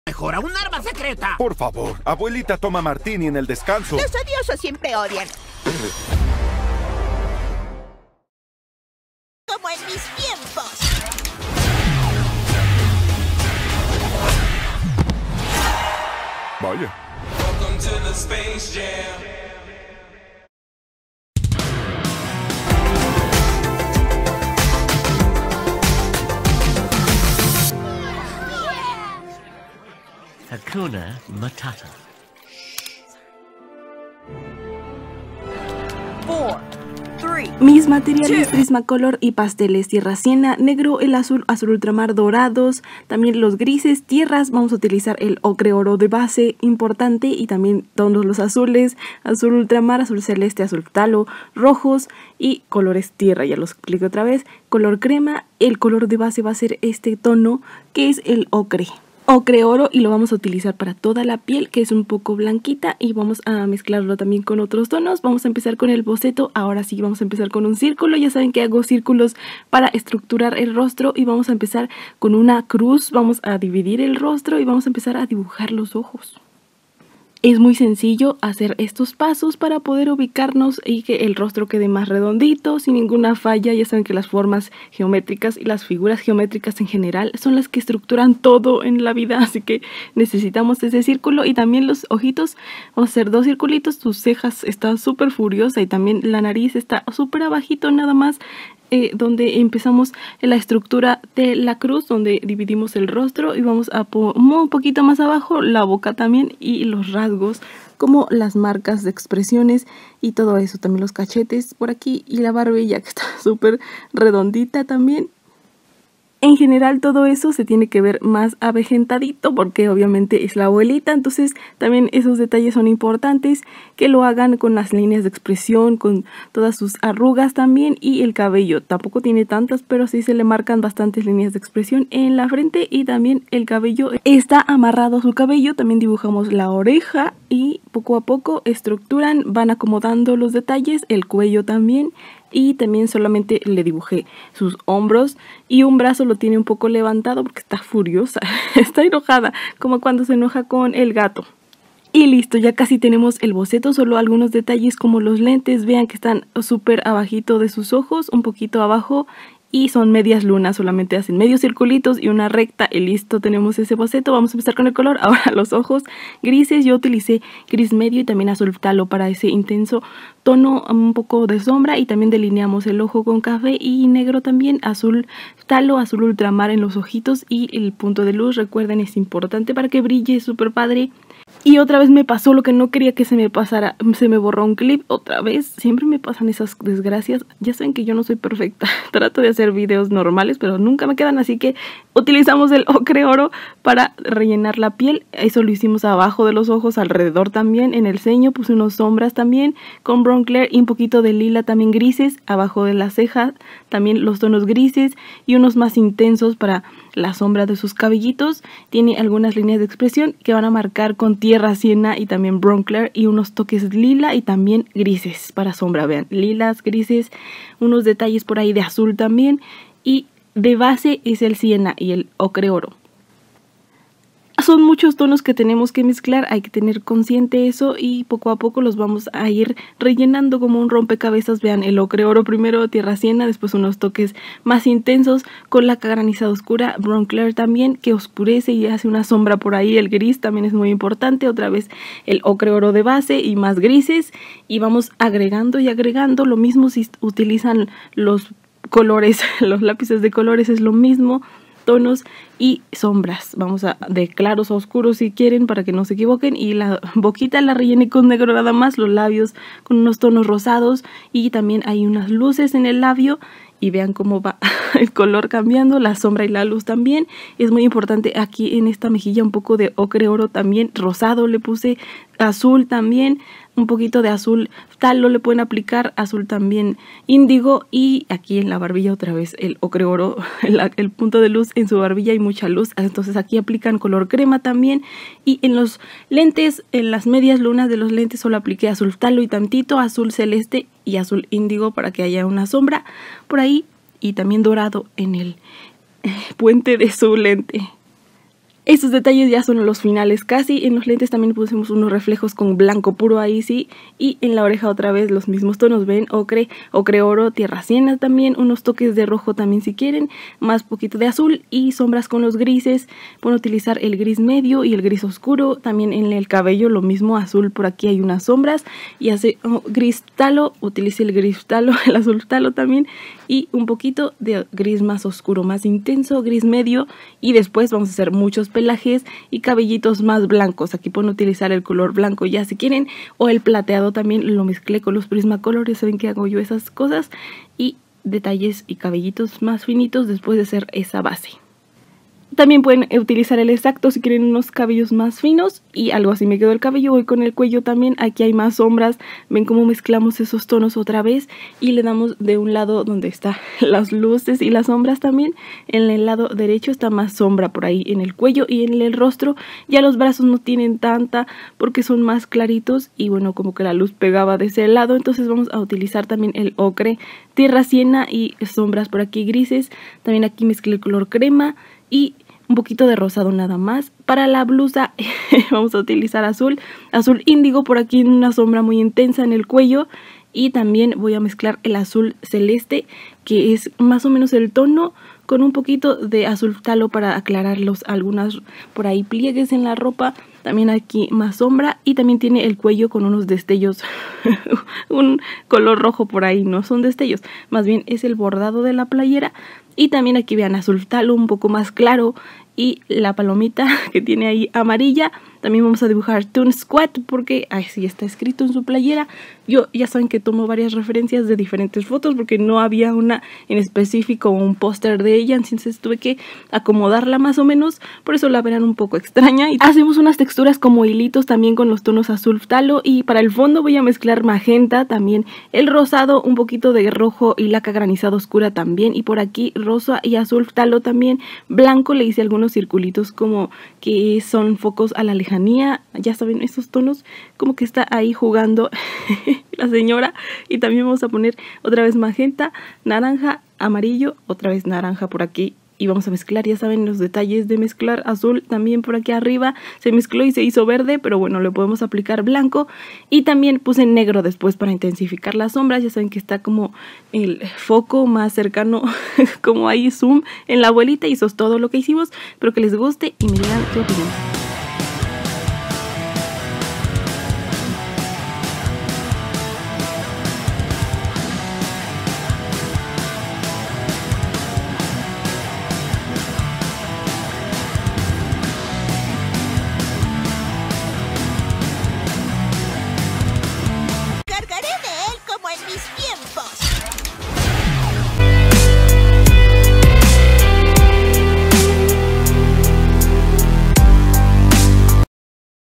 DIBUJO A ABUELITA SPACE JAM (LOONEY TUNES) HOW TO DRAW GRANDMA SPACE JAM NEW LEGACY
APRENDE A DIBUJAR CON MIS TECNICAS DE DIBUJO TODOS LOS PERSONAJES DE LA PELICULA  SPACE JAM LOONEY TUNES  CON COLORES Y MATERIALES FACILES DE MANEJAR COMO UN PROFESIONAL

MATERIALES : PASTELES REMBRANT Y PAN PASTEL , PRISMACOLOR PREMIER , PAPEL FABRIANO BLANCO GRAMAGE 160 MARCA RODIN